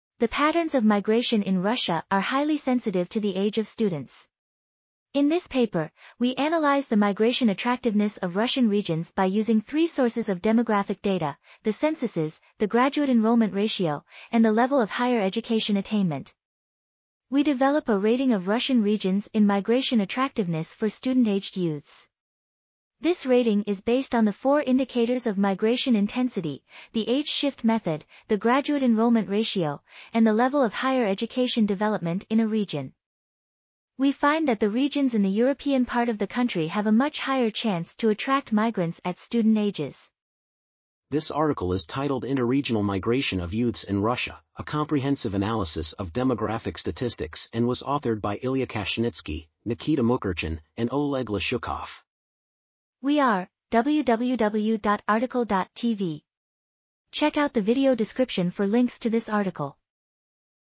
Interregional Migration of Youths in Russia: A Comprehensive Analysis of Demographic ... | RTCL.TV
### Keywords ###

### Article Details ###
Title: Interregional Migration of Youths in Russia: A Comprehensive Analysis of Demographic Statistics
Authors: Ilya Kashnitsky, Nikita Mkrtchyan ,and Oleg Leshukov
Publisher: National Research University Higher School of Economics (HSE)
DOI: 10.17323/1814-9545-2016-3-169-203

### Video Timestamps ###
0:00:00 - Summary
0:00:40 - Title
0:00:52 - Outro
0:01:00 - End